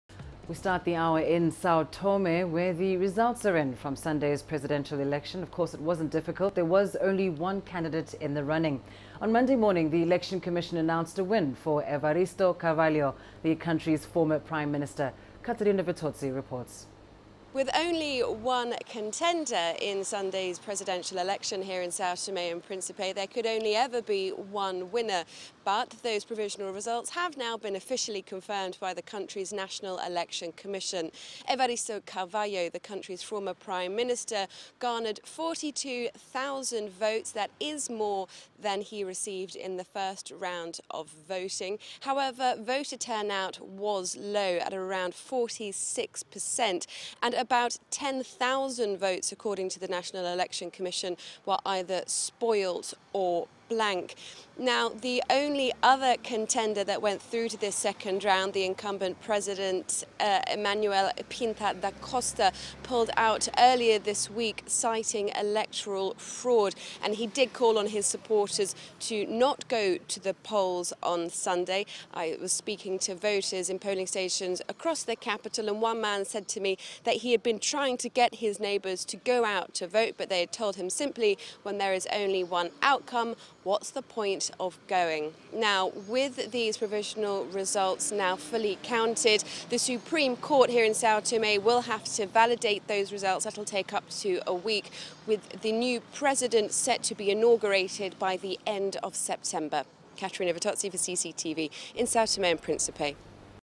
Evaristo Carvalho wins Sao Tome presidential elections
Results are in from Sunday's Sao Tome and Prencipe presidential election. There was only one candidate in the running. On Monday morning, the election commission announced a win for Evaristo Carvalho, the country's former prime minister